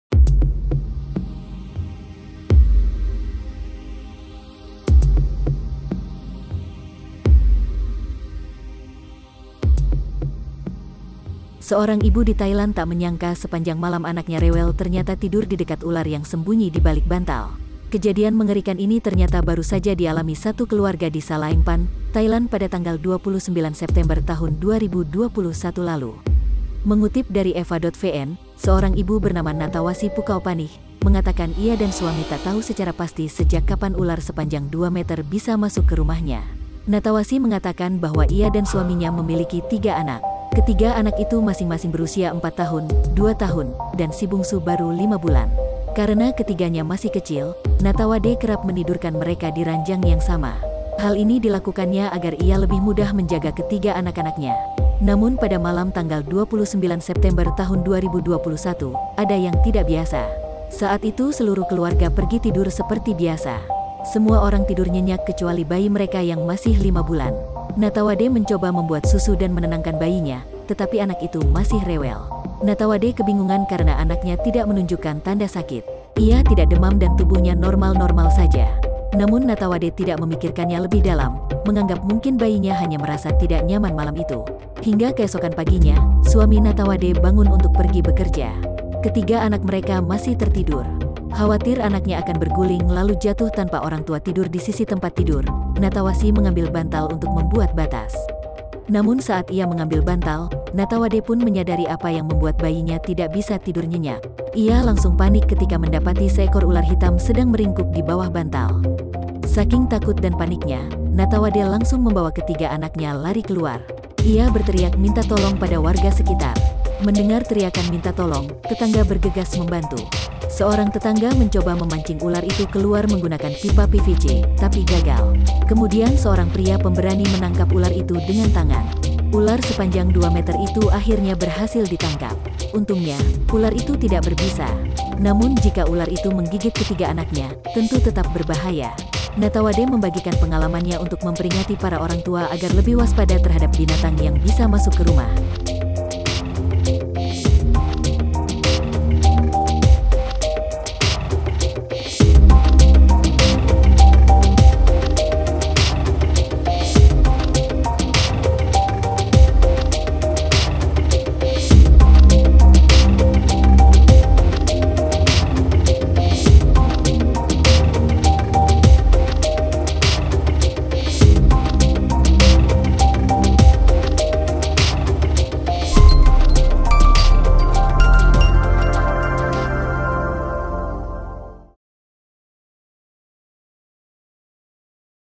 Tiap Malam Anak Tak Tidur Nyenyak Ternyata Ular Hitam di Bawah Bantal
Seorang ibu di Thailand tak menyangka sepanjang malam anaknya rewel ternyata tidur di dekat ular yang sembunyi di balik bantal.

Kejadian mengerikan ini ternyata baru saja dialami satu keluarga di Salaeng Phan, Thailand pada 29 September 2021 lalu.

Mengutip dari eva.vn, seorang ibu bernama Nattawasee Phukaopanich (32), mengatakan ia dan suami tak tahu secara pasti sejak kapan ular sepanjang 2 meter bisa masuk ke rumahnya.